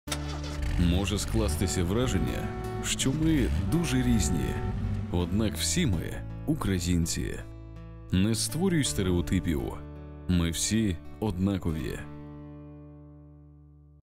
Інфокампанія від ВГО «Магнолія» «Не створюй стереотипів, ми всі однакові!» 3
Проект "Сприяння плюралізму та діалогу в українському суспільстві" спрямований на підвищення ролі активних молодих представників громадянського суспільства задля розбудови відкритого, сильного, згуртованого, демократичного і плюралістичного суспільства в Україні.
В рамках проекту ВГО «Магнолія» розробило інфокампанію, що закликає українців пам'ятати, що ми всі з однієї країни, показуючи це у дуже креативний спосіб через бачення автовласників.

Проект реалізується Фондом Східна Європа у співпраці з німецькою Громадською організацією ChildFund Deutschland e.V. за рахунок коштів, наданих ChildFund Deutschland та Міністерством закордонних справ Федеративної Республіки Німеччини